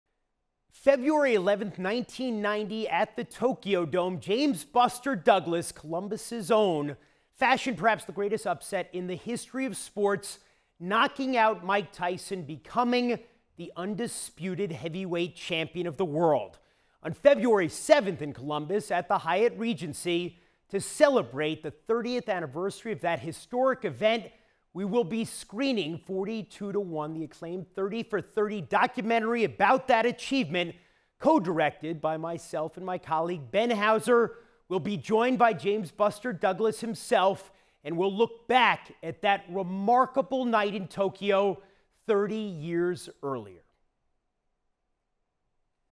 Jeremy Schaap PROMO for 42 to 1 Event
This is the first promotional announcement from ESPN for the James "Buster" Douglass 30th Anniversary Celebration to take place Friday, February 7, 2020 at the Hyatt Regency Hotel Ballroom. Join the conversation as award winning ESPN producers, Ben Houser and Jeremy Schaap share Buster Douglas stories from the making of their acclaimed documentary, 42 to 1, and take your questions. Go to the BuckeyeBusterDouglas website for additional information and find out how you can purchase your table for this exciting event!